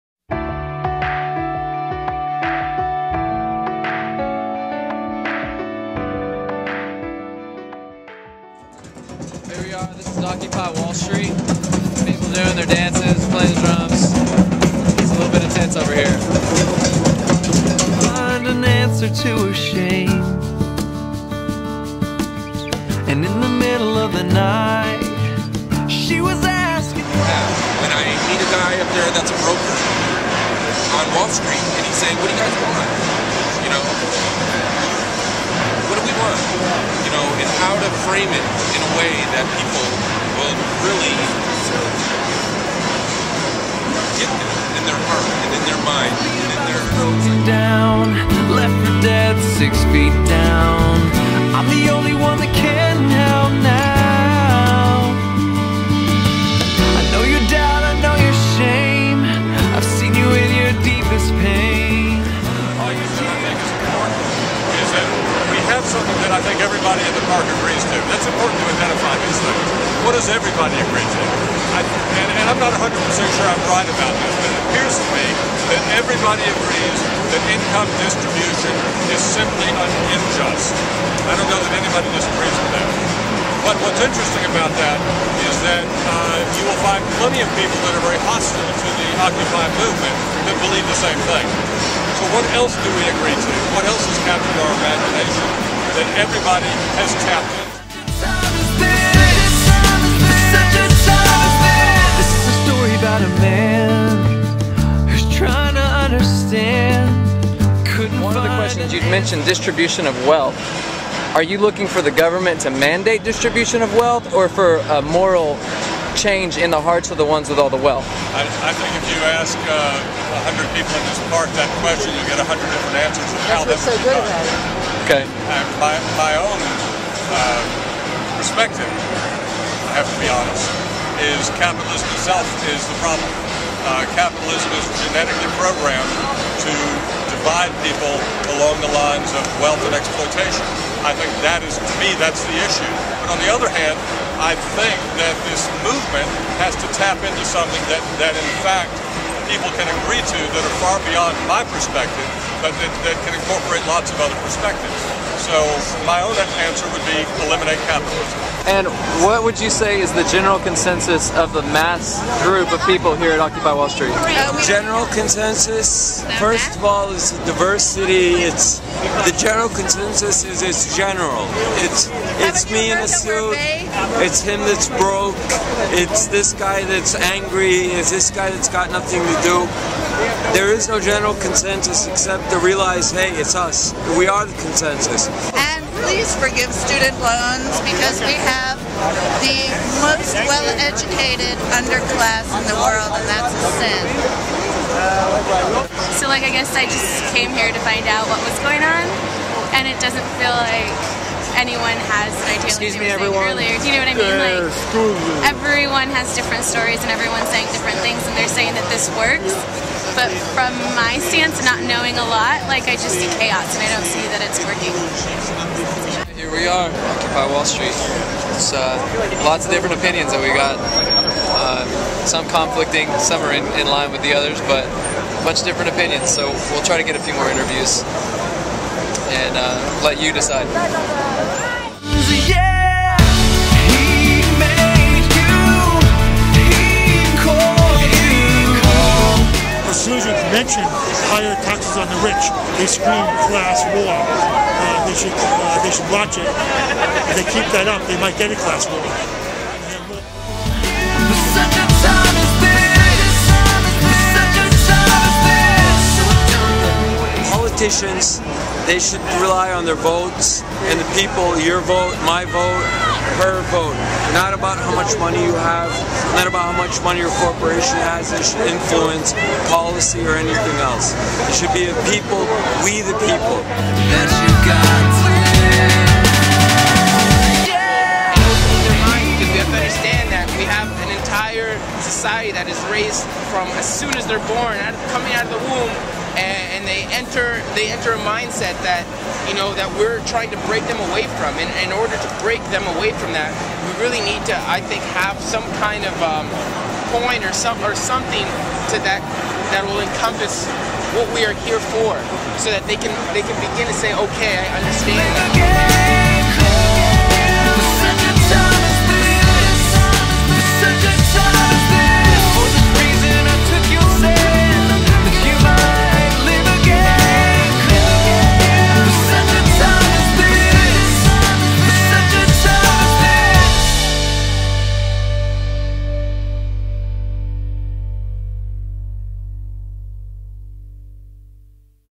Occupy Wall Street New York City October 2011
I interview "Occupy Wall Street" protestors on Halloween day.